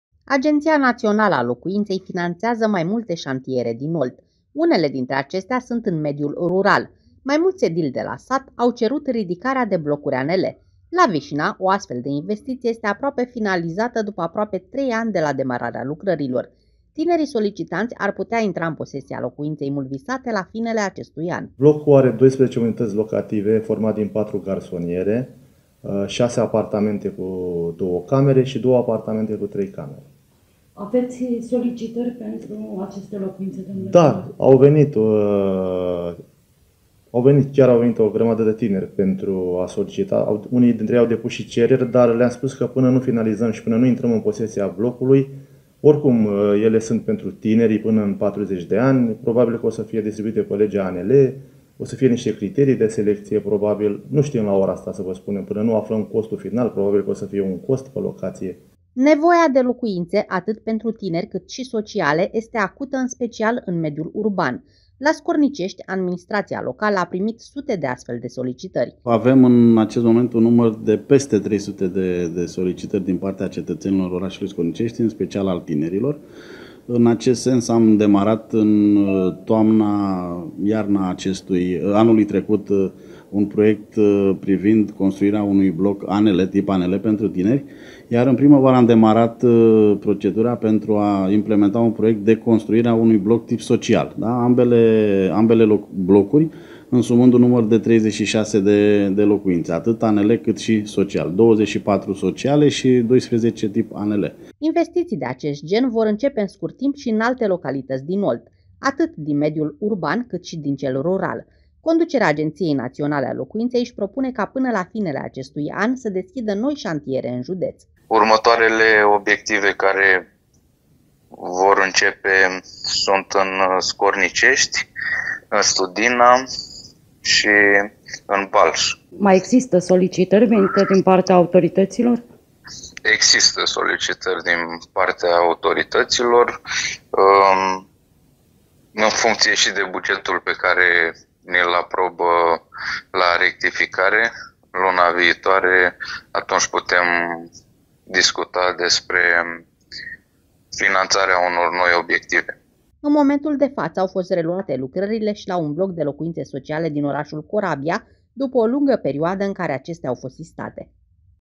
Construirea de locuințe pentru tineri, reluată
Asaltate de solicitările venite din partea tinerilor care vor o locuință, autoritățile din Olt au făcut demersuri pentru construirea de locuințe ANL. Astfel de locuințe se construiesc la acest moment atât în mediul urban, cât și în satele din județ. Primele locuințe pentru tinerii de la sat ar putea fi date în folosință la finele acestui an, în comuna Vișina.